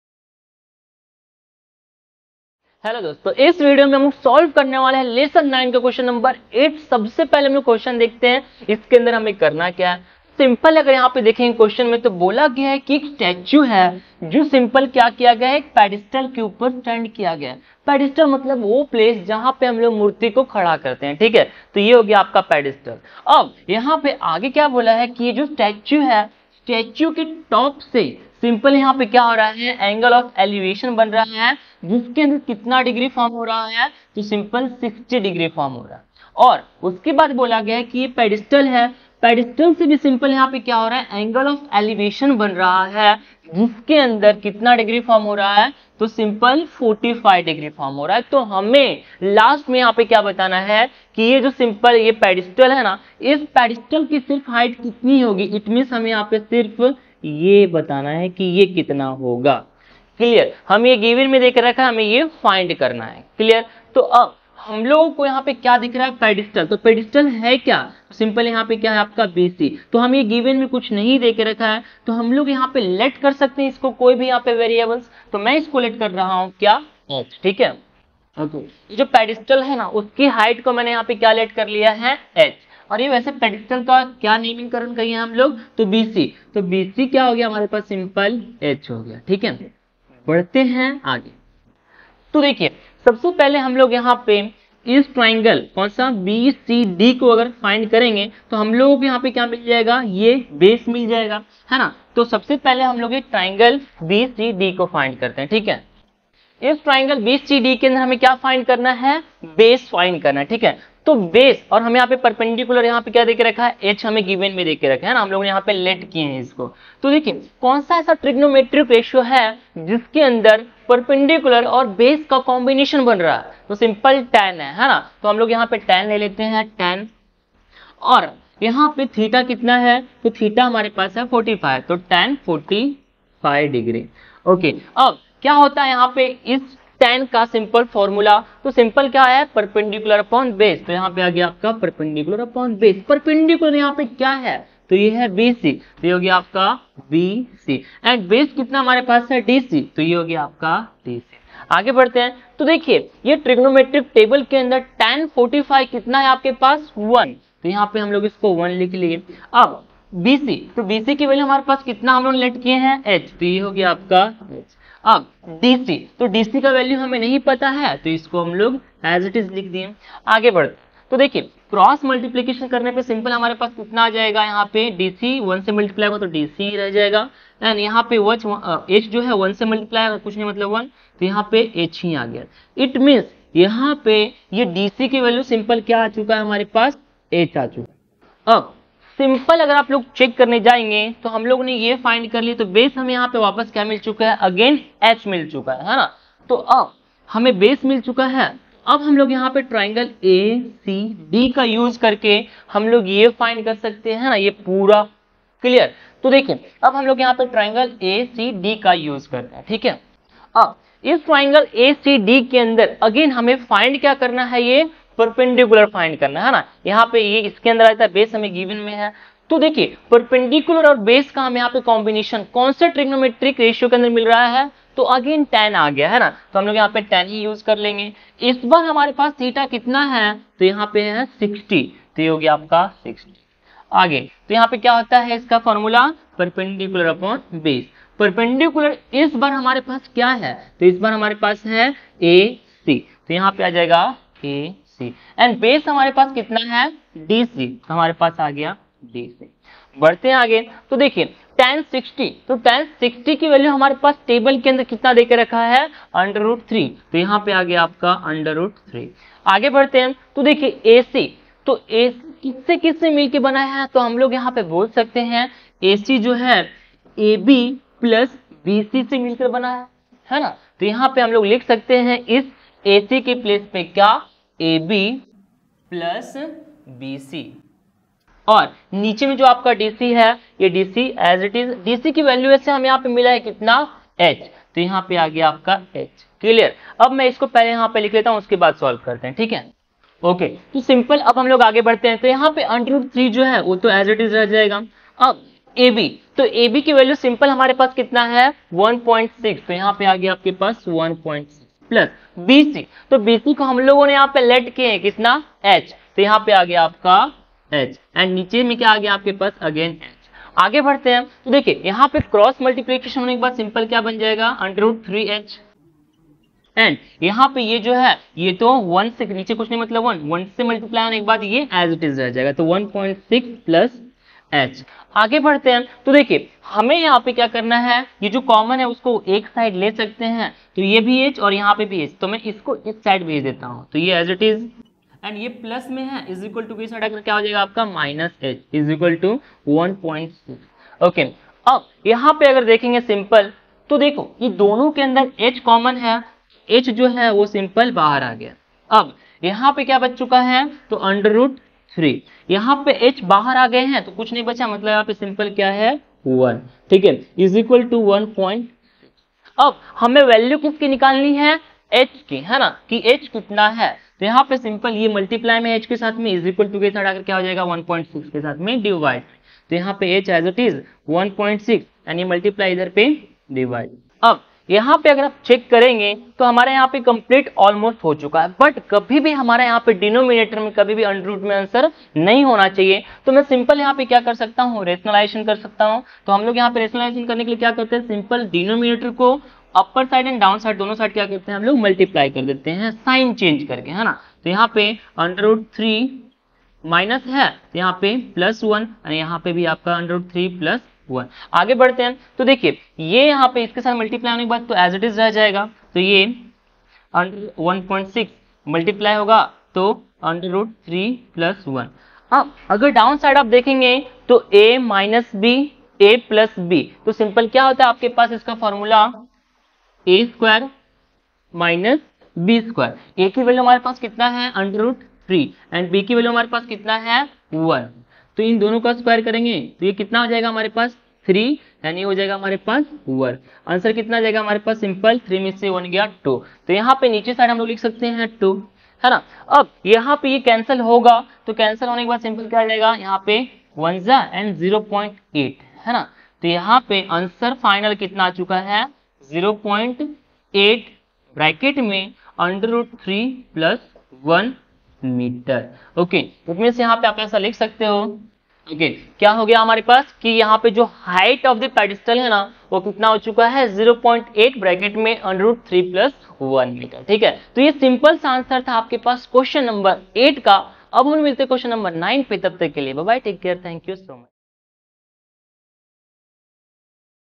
Class 10 Maths Chapter 9 | Exercise 9.1 Q-8 Solution | Some Applications of Trigonometry
🔍 Dive deep into Class 10 Maths Chapter 9 with our detailed solution! 📏 We tackle the intricacies of 'Some Applications of Trigonometry', providing a step-by-step guide to crack this specific problem. Enhance your understanding and ace your exams with our expert insights.

📘 Key Features:
- Clear explanation of the problem-solving process
- Application of trigonometric principles to real-world scenarios
- Time-saving tips and tricks for efficient problem-solving

🌟 About Us:
GURUKUL is your ultimate destination for in-depth educational content.

0:00 Intro
0:10  Question
1:03 Answer
7:53 Outro